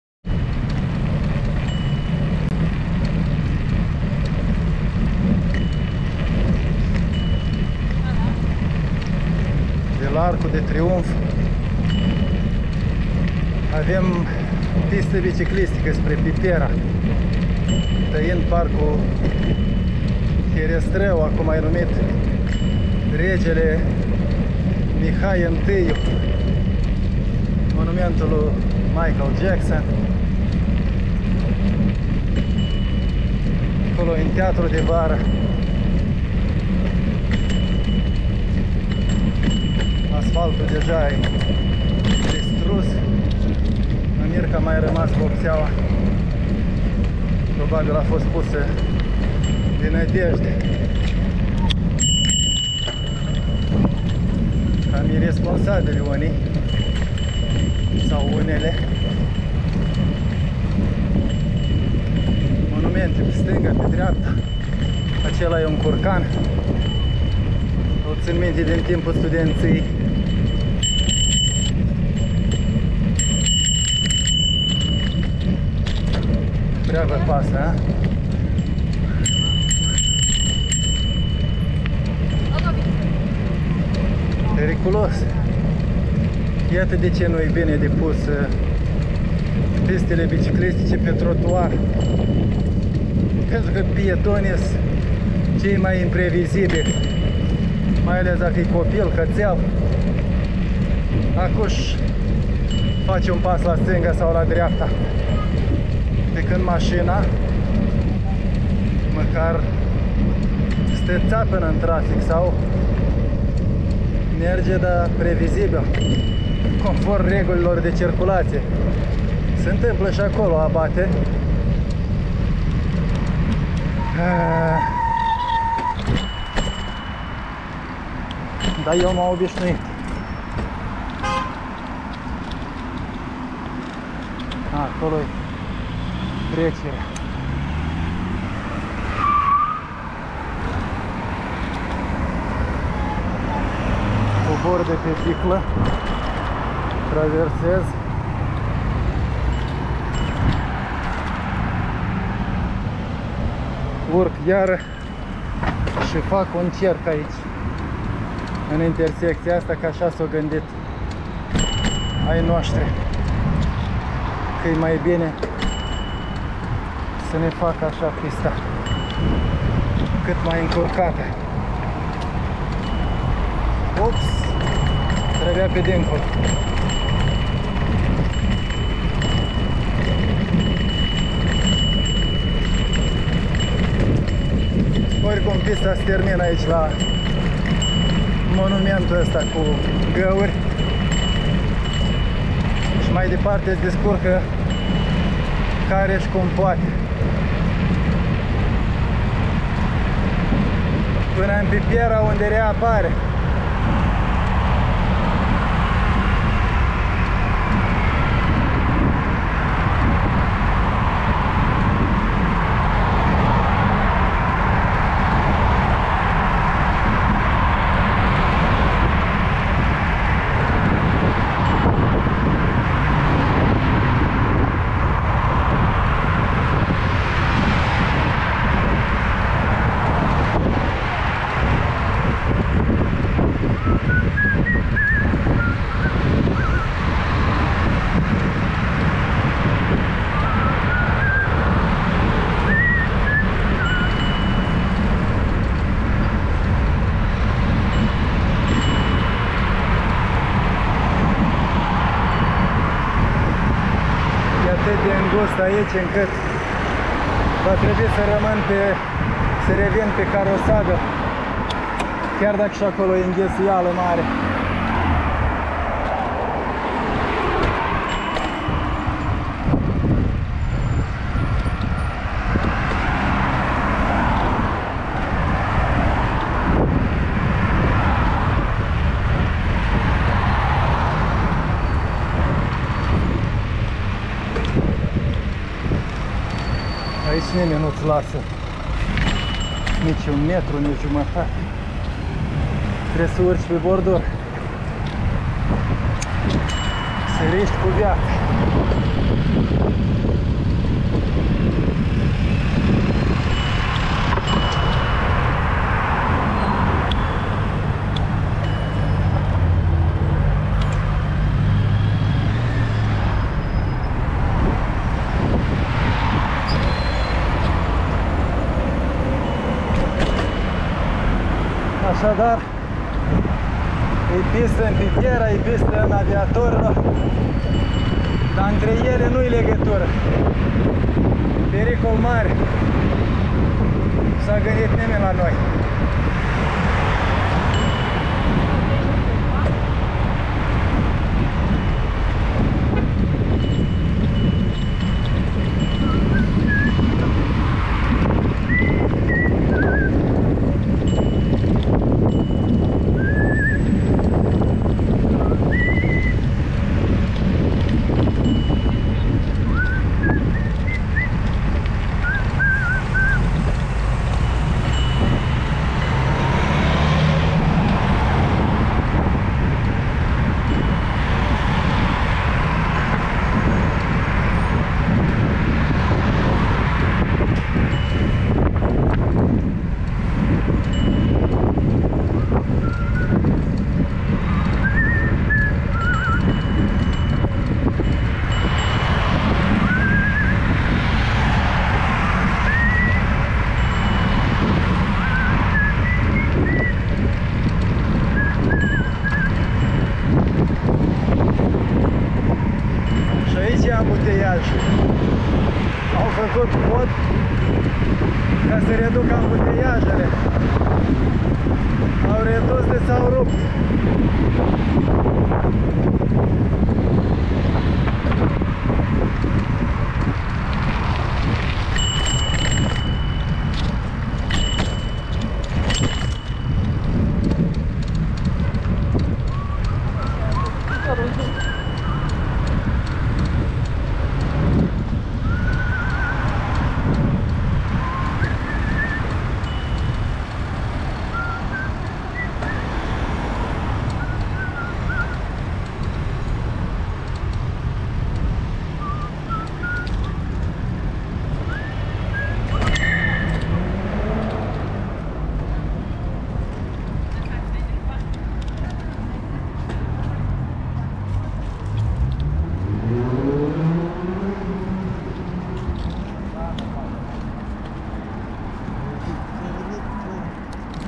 De la Arcul de Triumf spre Pipera ai pista biciclistică întreruptă - Curaj.TV
Dacă vrei să traversezi lacurile Herestrău de la Arcul de Triumf spre Nordul capitalei, poți s-o iei prin parcul Mihai 1 (așa se numește acum fostul parc Herestrău), e o pistă biciclistică veche, dar practicabilă. Atenție, totuși, sunt mulți pietoni neatenți! Apoi ai de traversat bulevardul Aviatorilor, apoi pe la poduri pista dispare, ca să re-apară în Pipera.